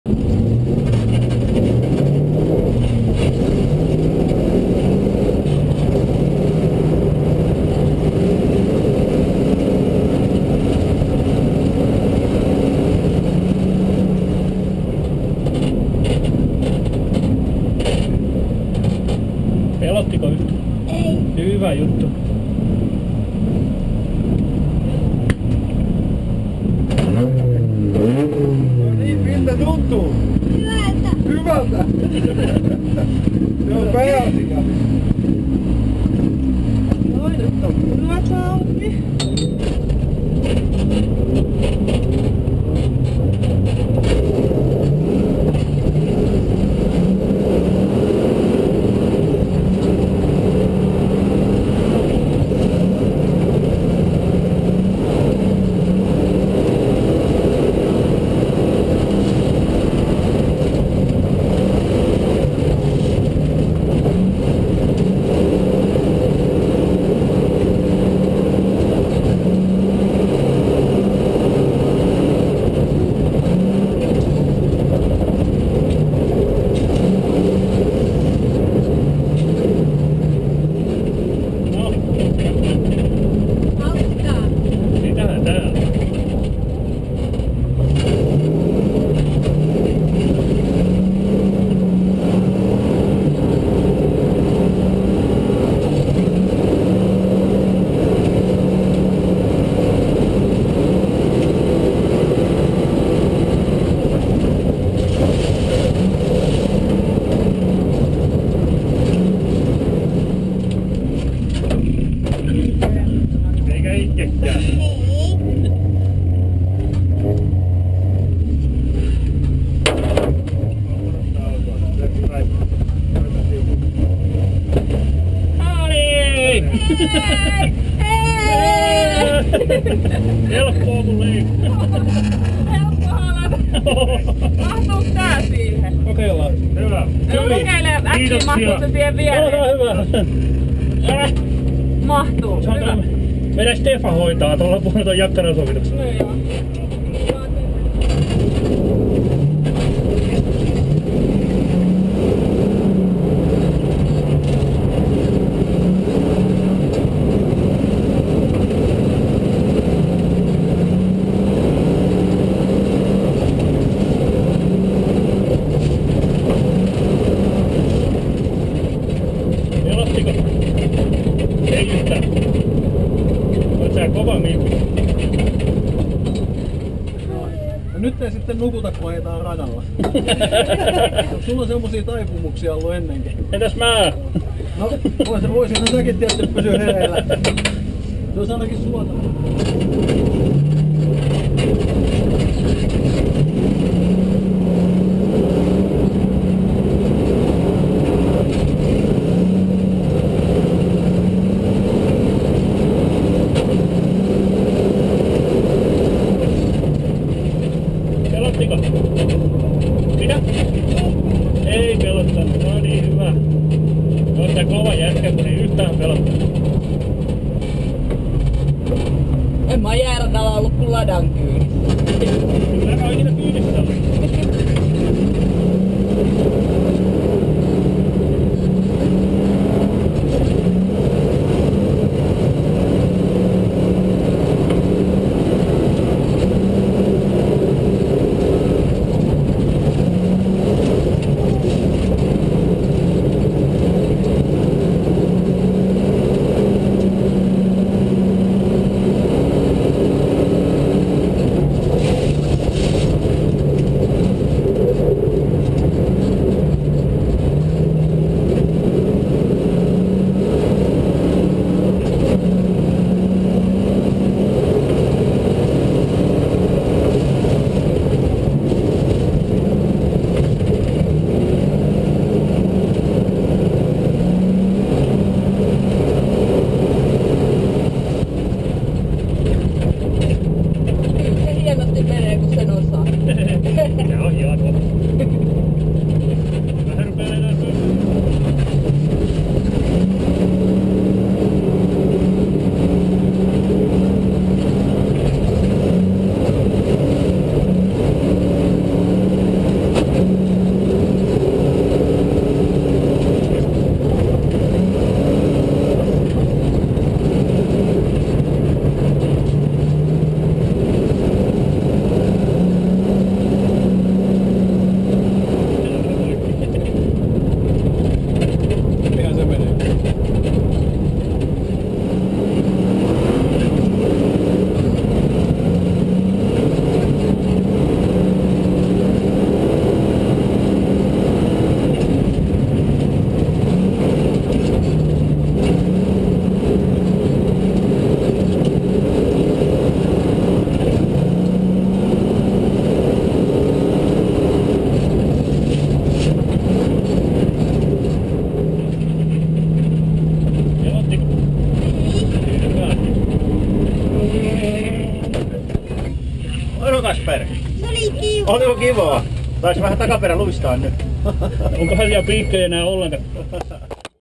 Rallisprint tähdet on ice
Suomen rallisprint tähdet on ice hyväntekeväisyys tapahtuma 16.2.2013 Ikaalisissa. Hyttikamera Pony Express Racing Ford Mustang GTS